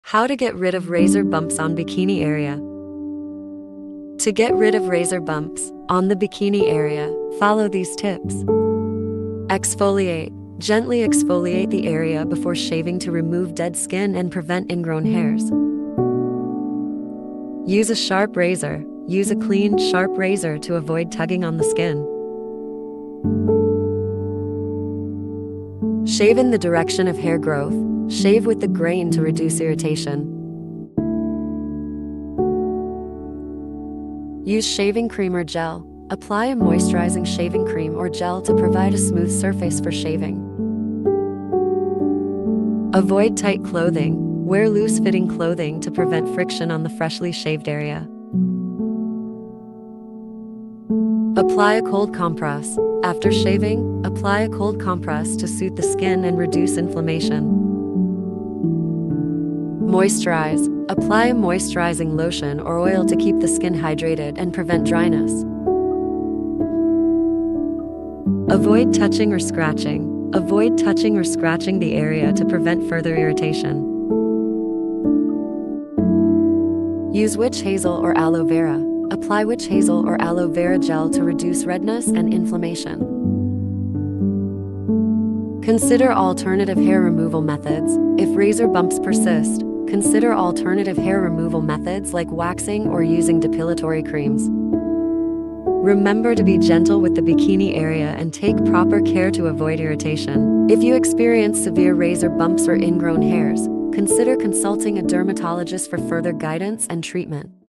how to get rid of razor bumps on bikini area
Discover effective tips to eliminate razor bumps on the bikini area in this helpful video. Learn how to exfoliate properly, use a sharp razor, shave with the grain, and apply moisturizing products for a smoother shave. Explore post-shave remedies like cold compresses and soothing creams to reduce redness and inflammation. Find out how to prevent irritation by wearing loose clothing and avoiding scratching.

Enhance your bikini area care routine with witch hazel or aloe vera for added comfort. Say goodbye to razor bumps and achieve a bump-free bikini area with these simple and practical methods.